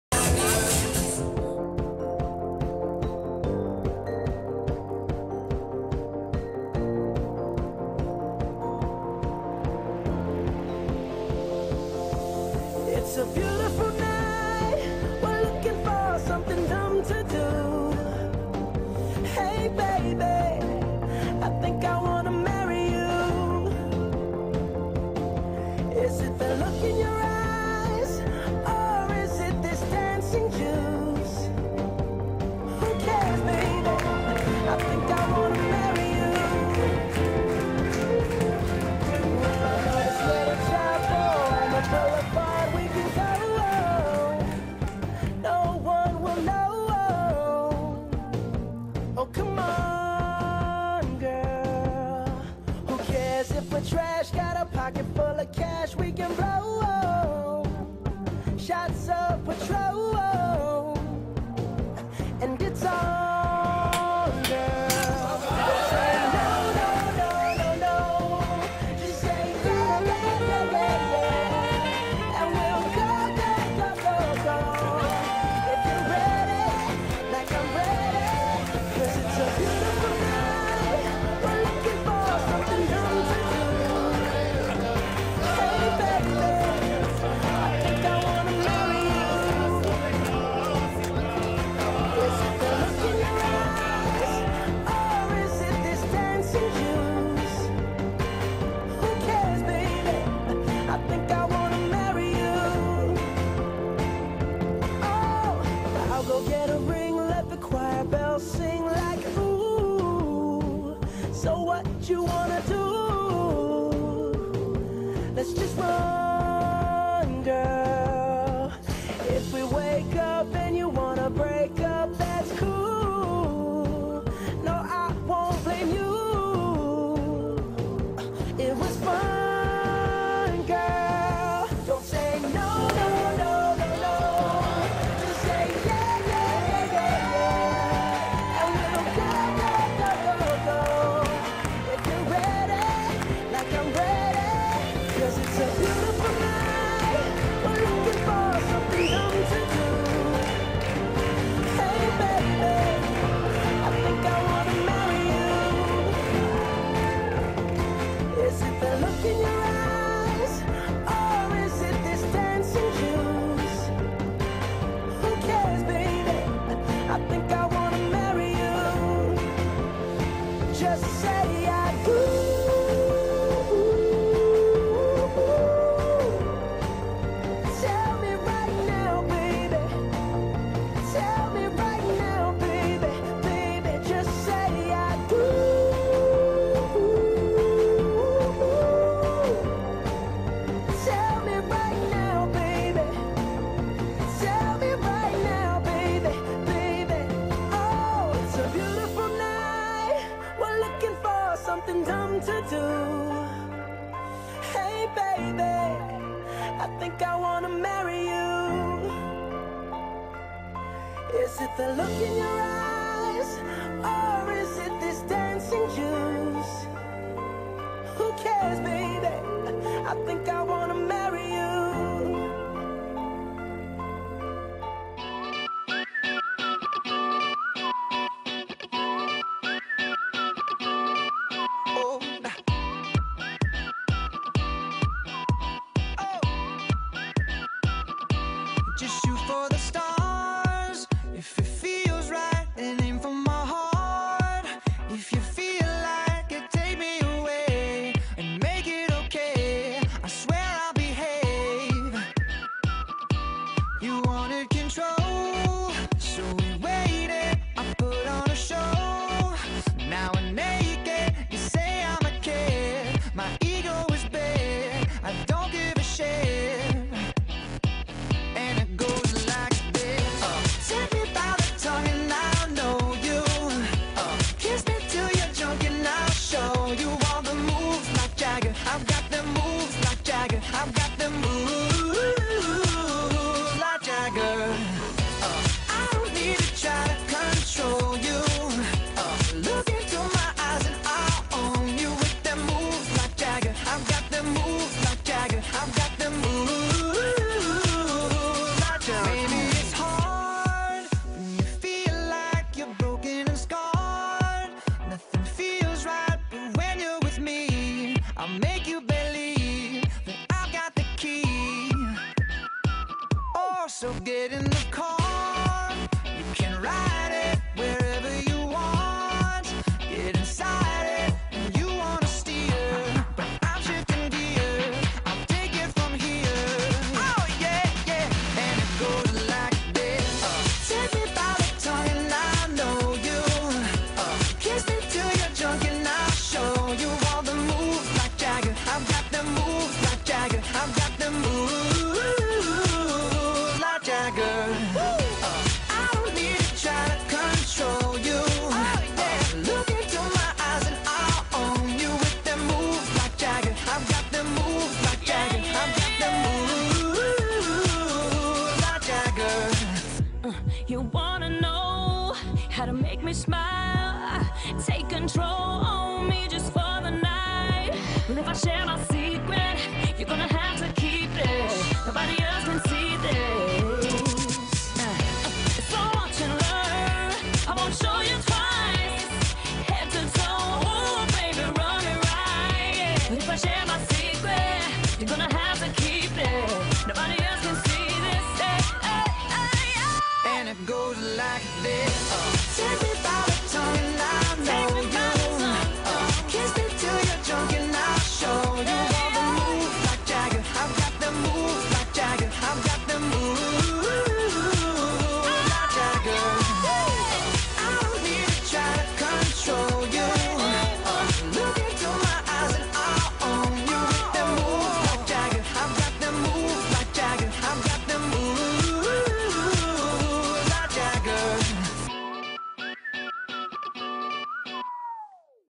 Modern Jewish Orthodox wedding @ BAYT
Modern Jewish Orthodox wedding at The BAYT in Thornhill, Ontario
View in HD: switch YouTube quality to 1080p